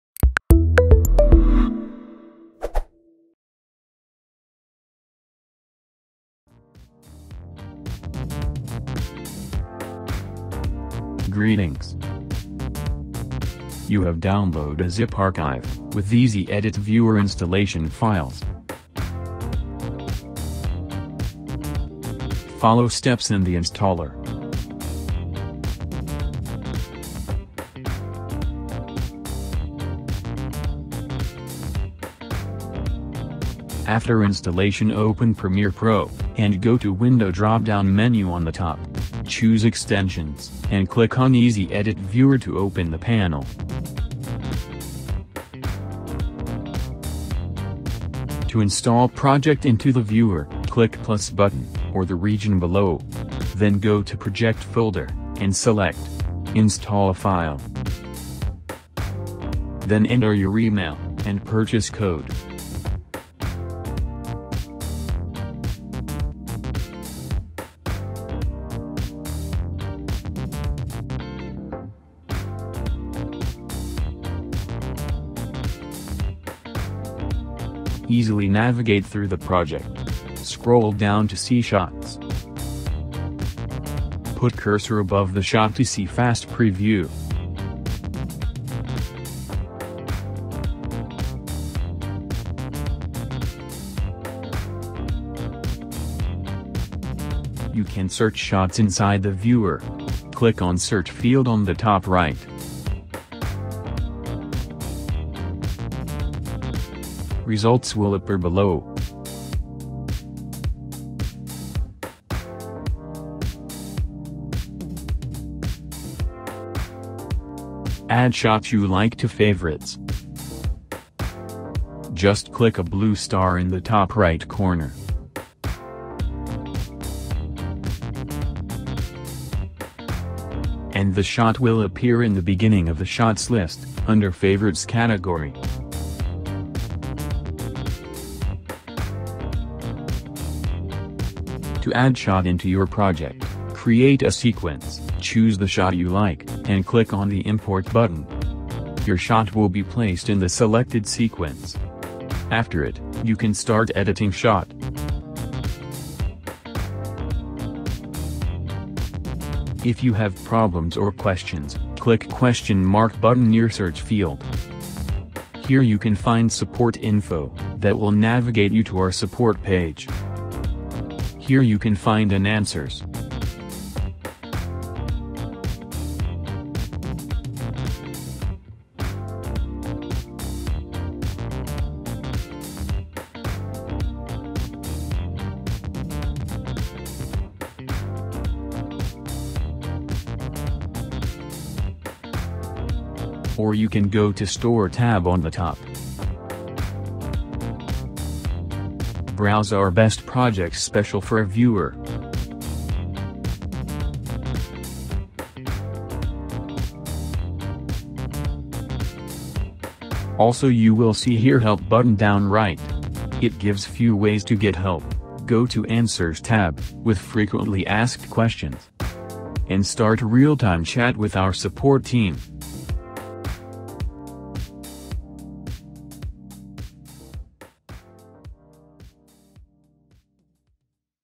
EasyEdit Viewer - Free Extension for Premiere Pro - How to use plugin
EasyEdit Viewer is Now Available for Premiere Pro!🔥 Watch how to use it and make your work simple and fast!

It`s a free extension from EasyEdit Pro for you to make the work process in Adobe Premiere Pro easier. Using this free plugin you can find, preview and import any element you need in just a few seconds.
Work with templates like a pro😍